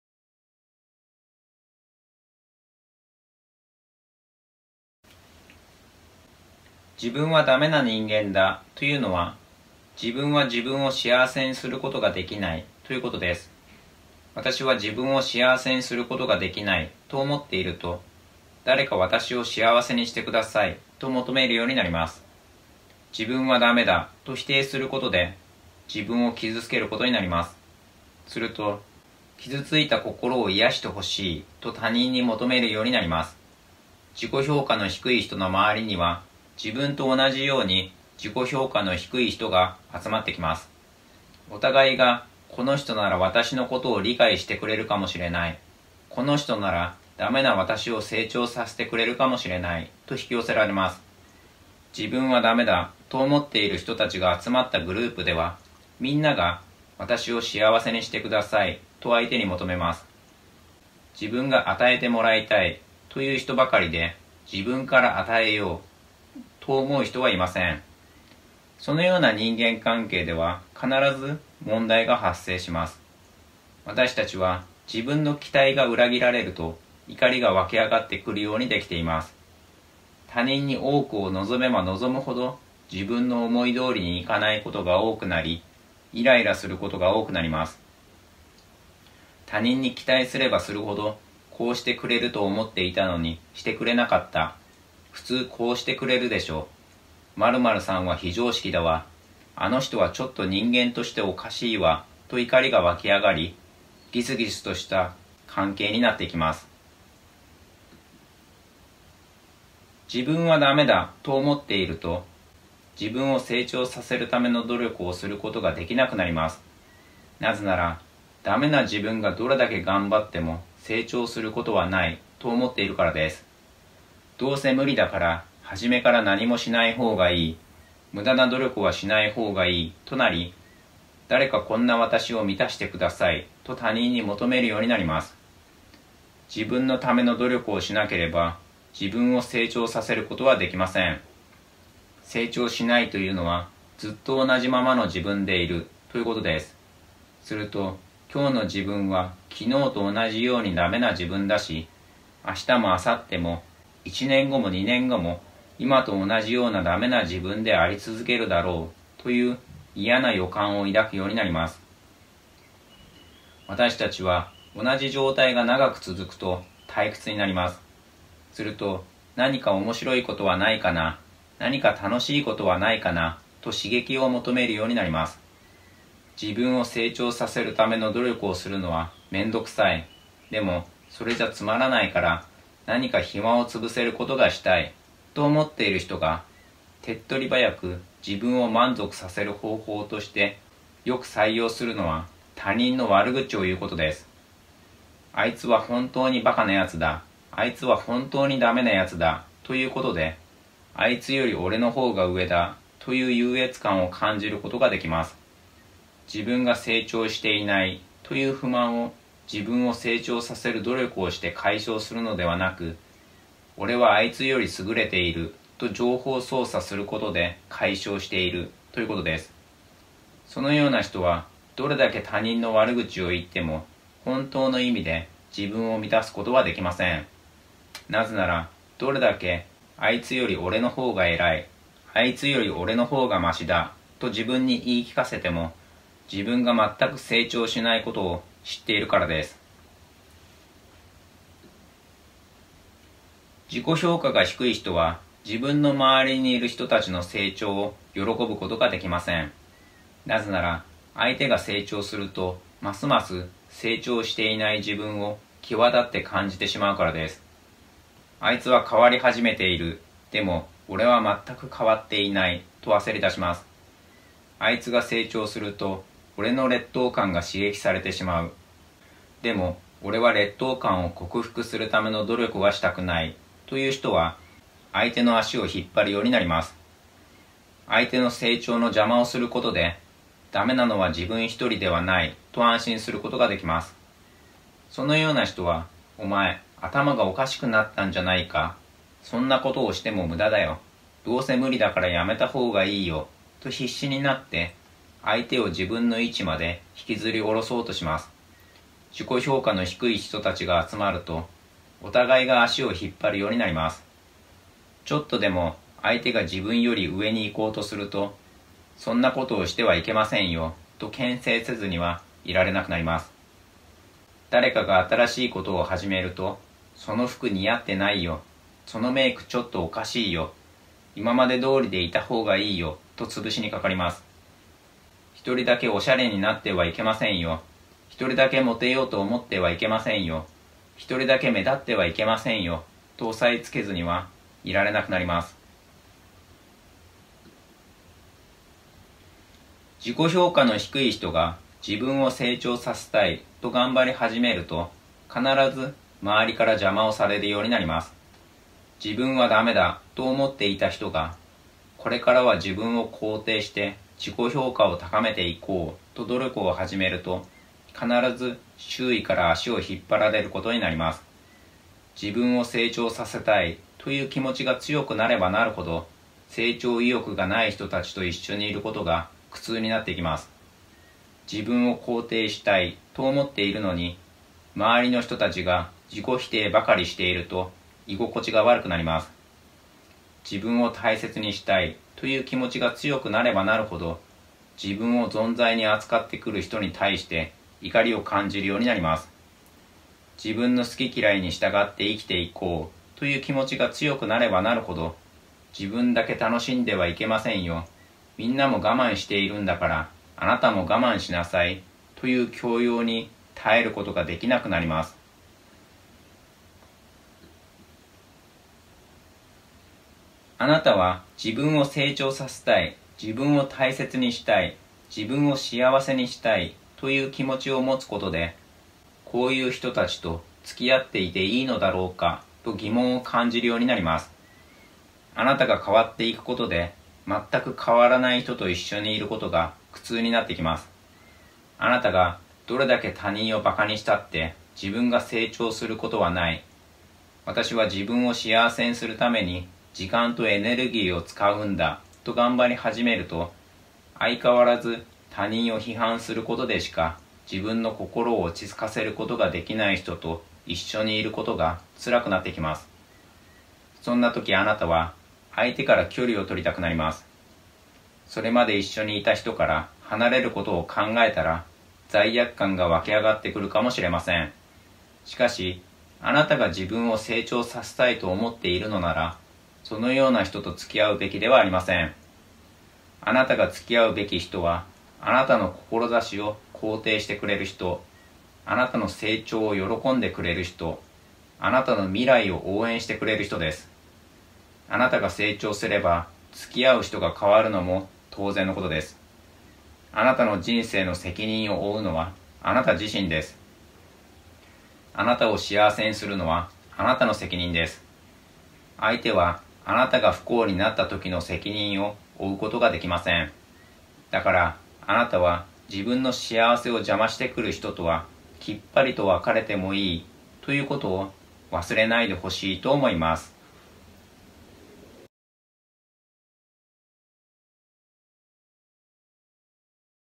【字幕付き】苦しい人間関係から抜け出そうとすると、罪悪感を感じる。だから、我慢してしまうんです！！！
▼えばっちの欲しいものリスト